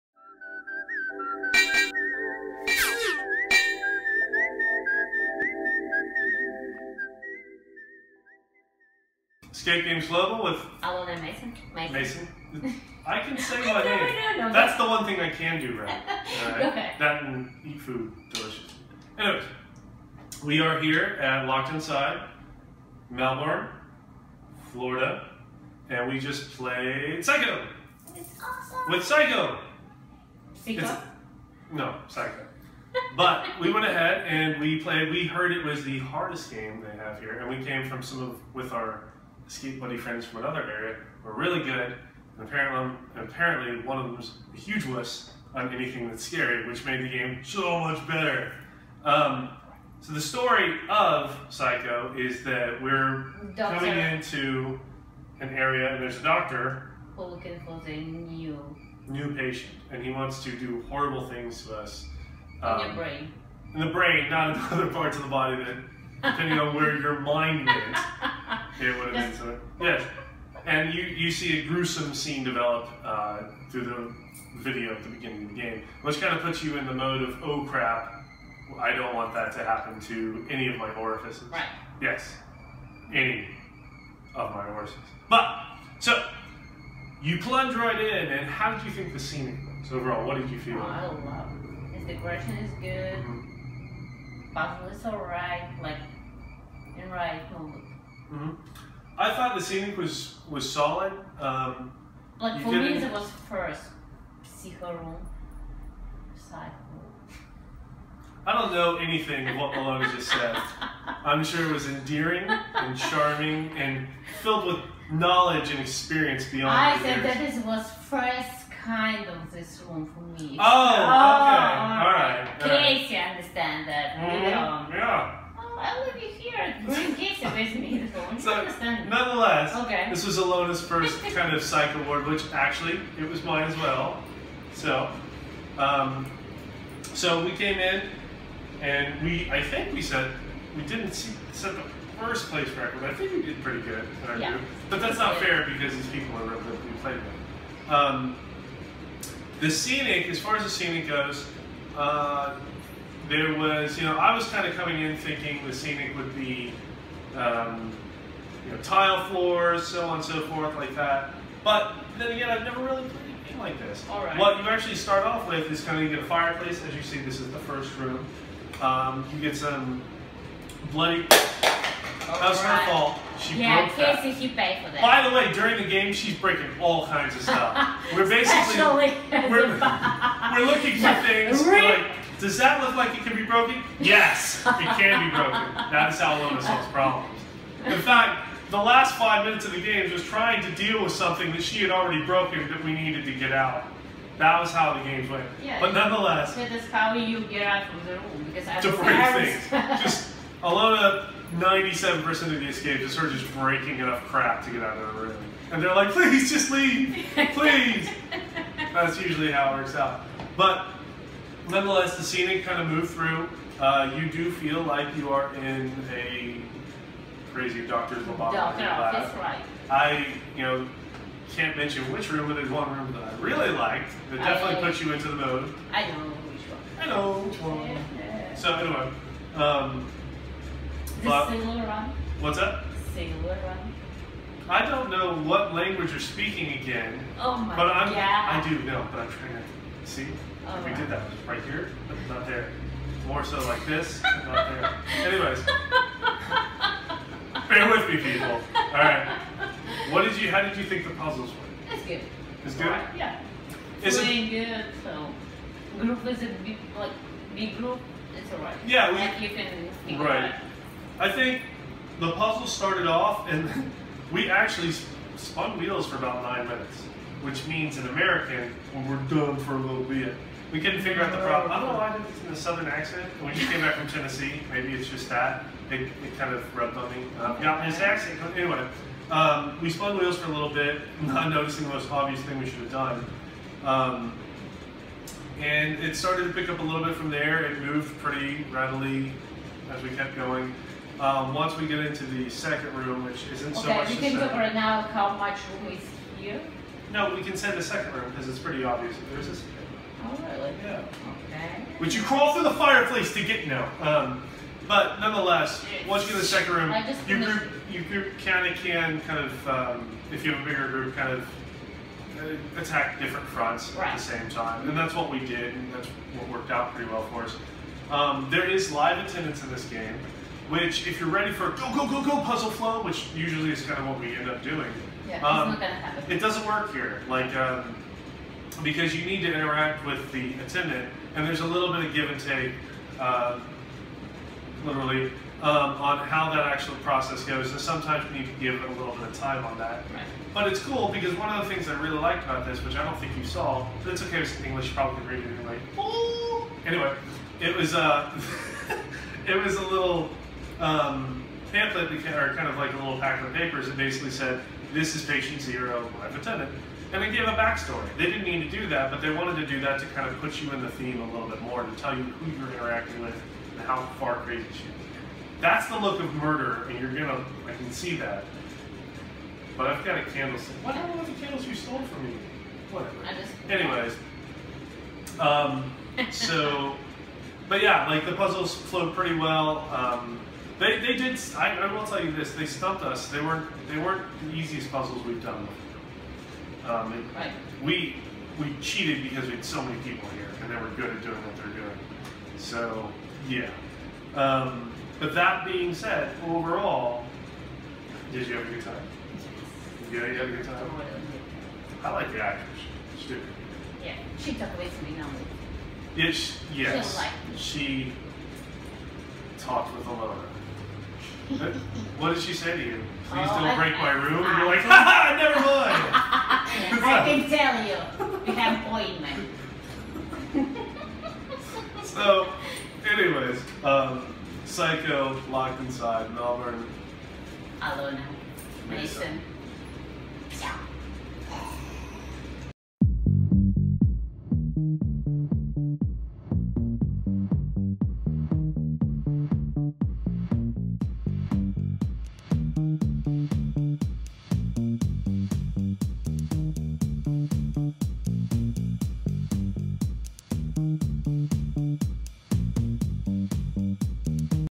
Pyscho, Locked Inside - Escape Game Review - Alona and Mason with Escape Games Global
Our friends at Locked Inside, in Melbourne, Florida allowed us to come and partake in a little crazy (as if Alona didn't already supply it on a daily basis). Nonetheless, we came, we saw, we got a little demented and we escaped (while Alona broke things). We had a great time and we'd like to tell you all about it...

Our game was comped by the awesome staff of Locked Inside. Thank you so much!